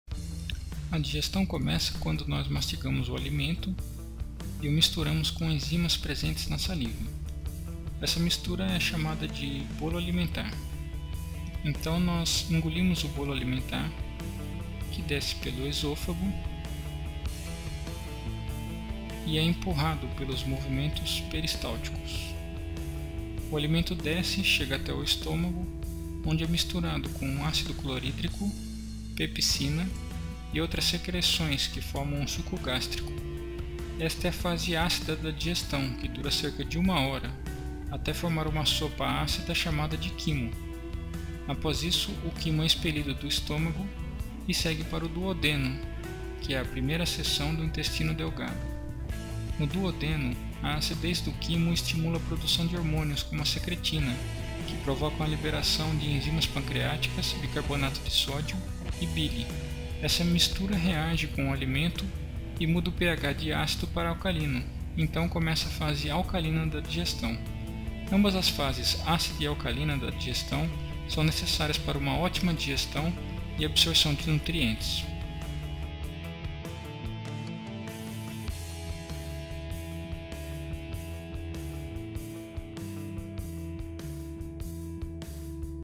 Como Ocorre a Digestão dos Alimentos em 3D
ELIMINE A AZIA E O REFLUXO EM 28 DIAS
Leia o livro impresso - MÉTODO ADEUS AZIA

Saiba como funciona a digestão em animação 3D.

Olá, sou Tegon Junior! Quero ajudar você a dizer adeus à gastrite, à azia e ao refluxo naturalmente! Neste vídeo vou mostrar para você como funciona a digestão dos alimentos em animação 3D.